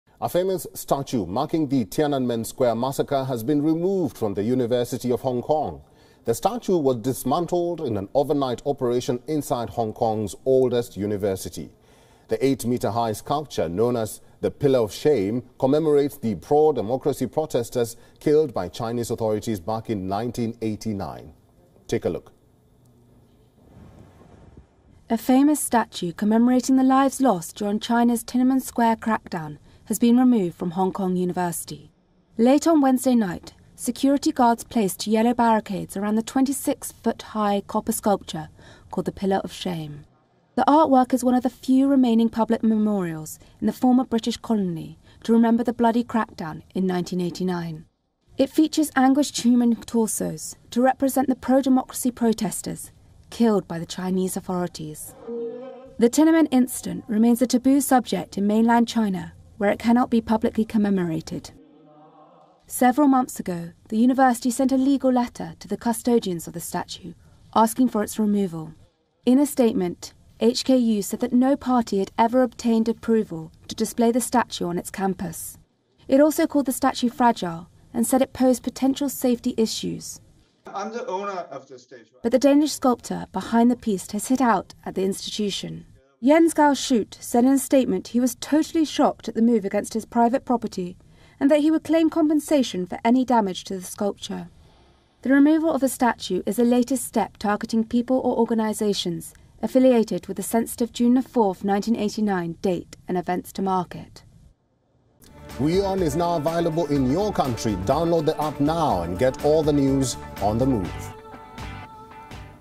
Hong Kong's oldest university removes Tiananmen square memorial statue from its campus | WION
In the latest blow to academic freedoms, Hong Kong's oldest university has removed the Tiananmen square memorial statue from its campus. The statue commemorated pro-democracy protesters killed by Chinese troops around Tiananmen square back in 1989.